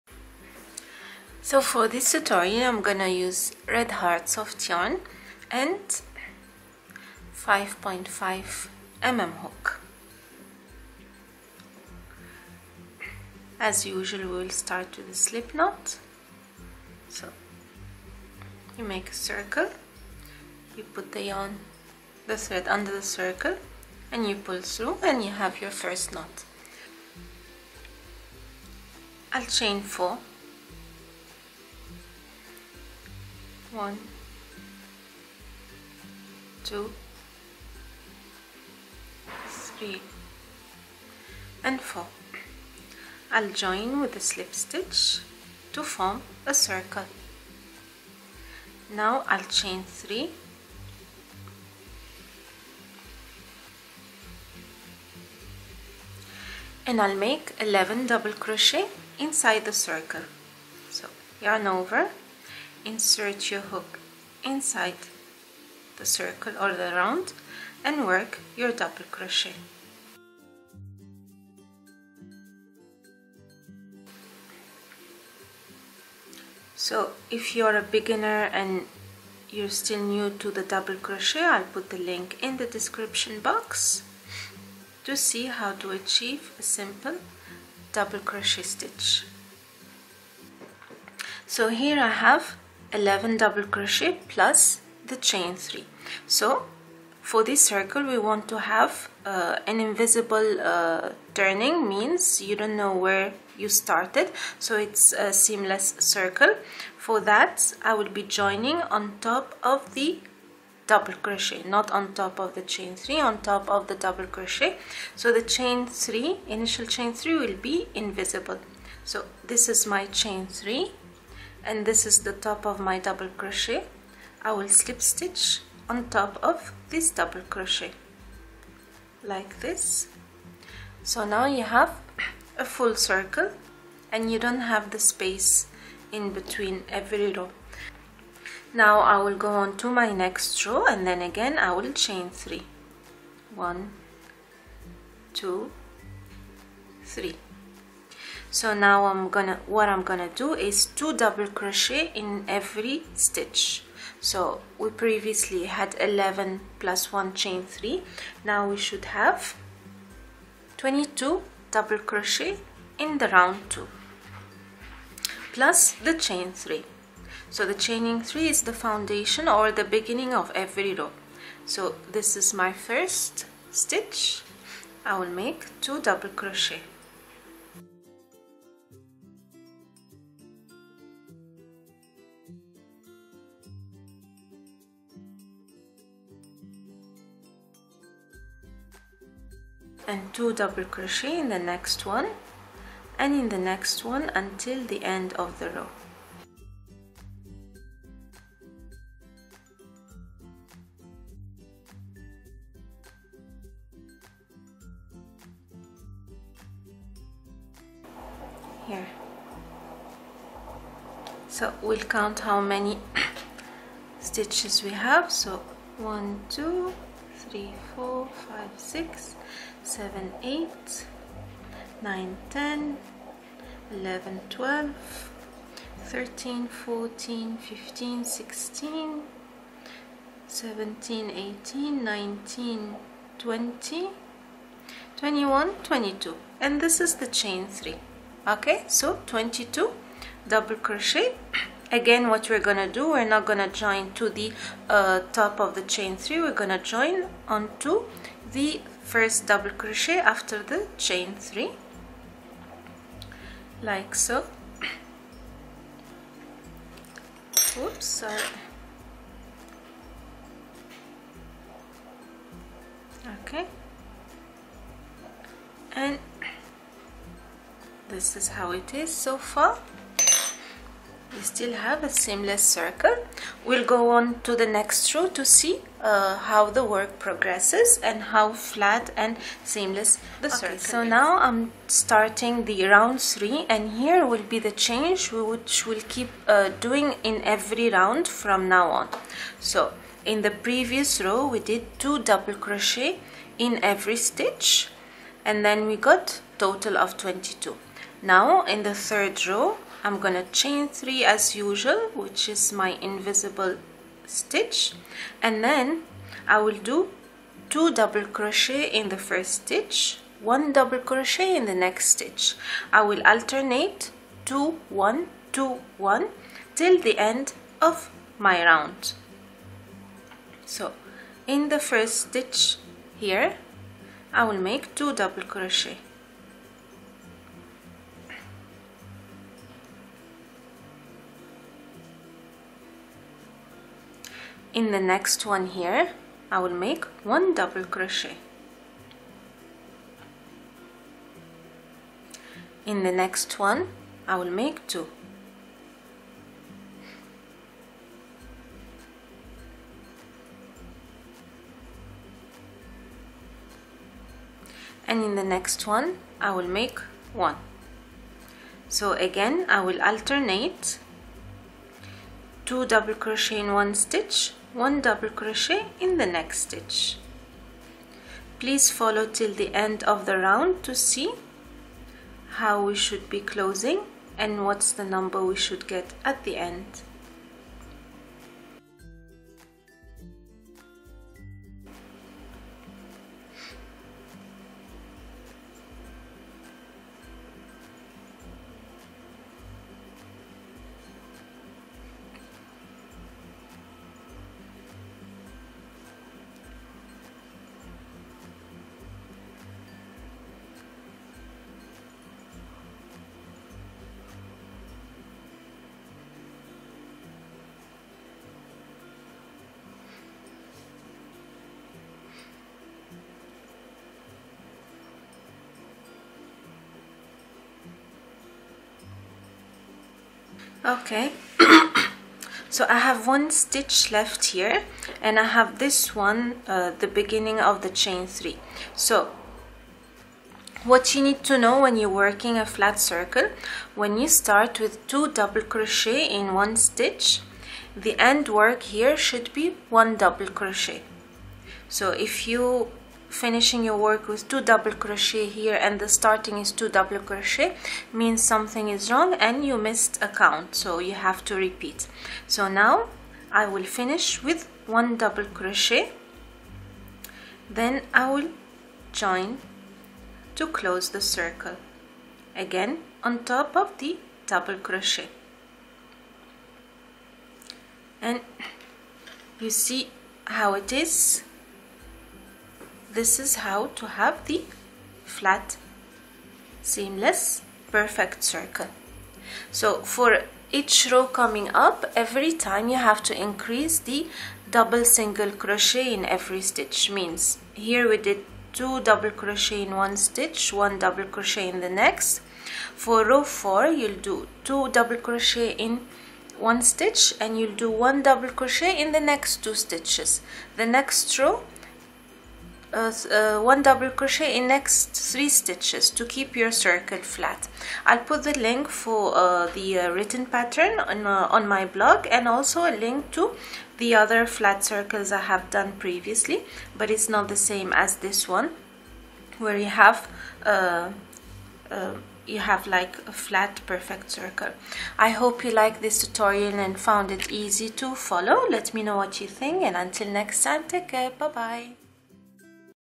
How to Crochet a perfect CIRCLE
As promised, here is another tutorial on how to crochet the perfect circle with an invisible ch to make work even better, enjoy the video :)

YARN USED: red heart yarn
CROCHET HOOK:  5.5 MM